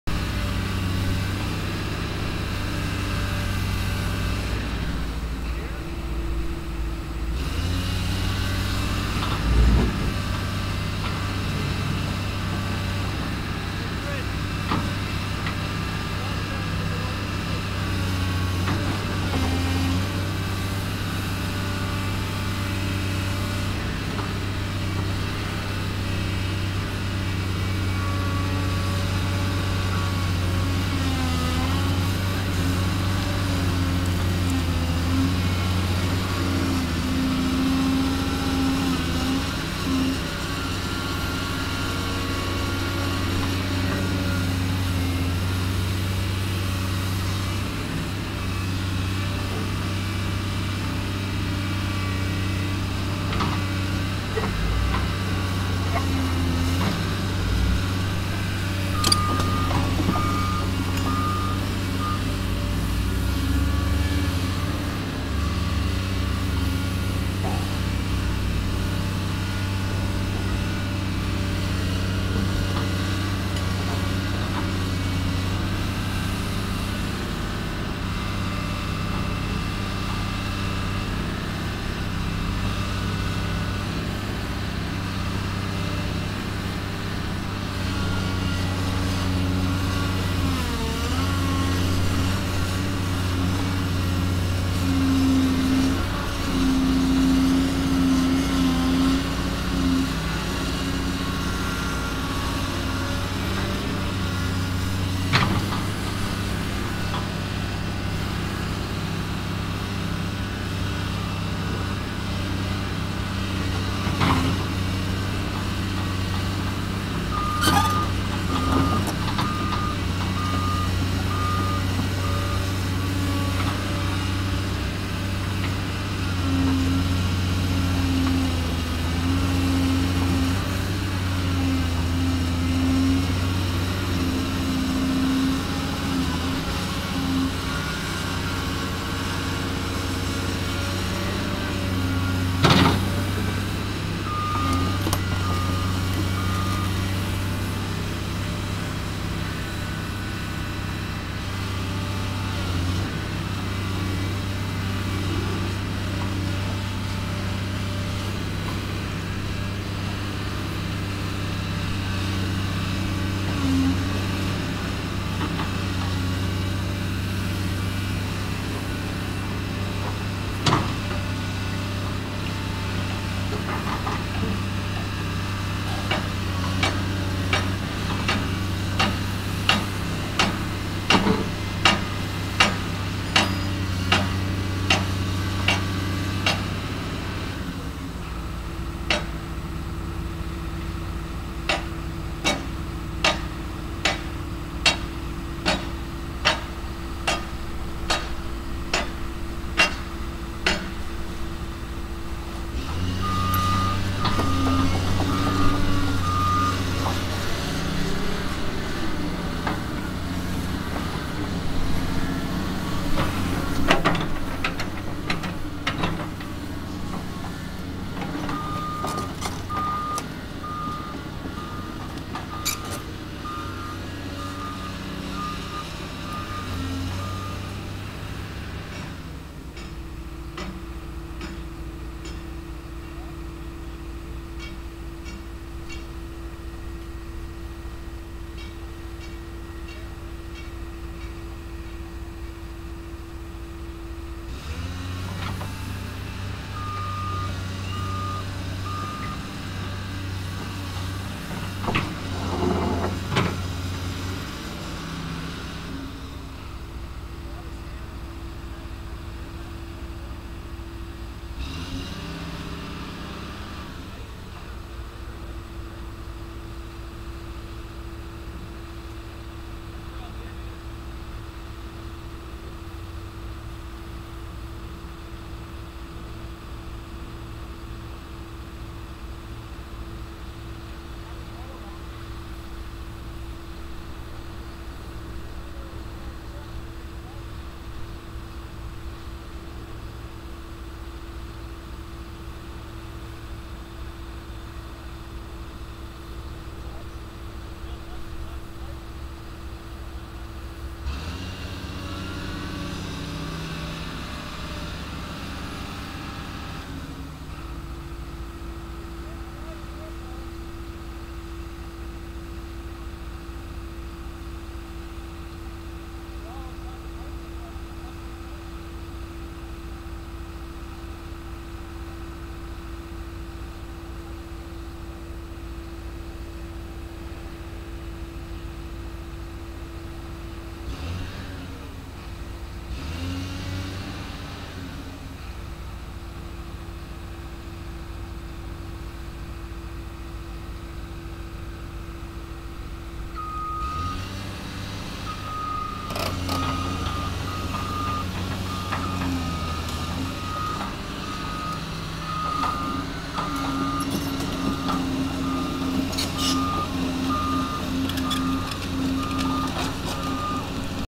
March 2, 2011 - Construction (Middle Sackville) (Part 2)
Tearing  up the old Sackville Road,  and making a road to Margeson Drive (which leads to Sackville Drive the Highway 101).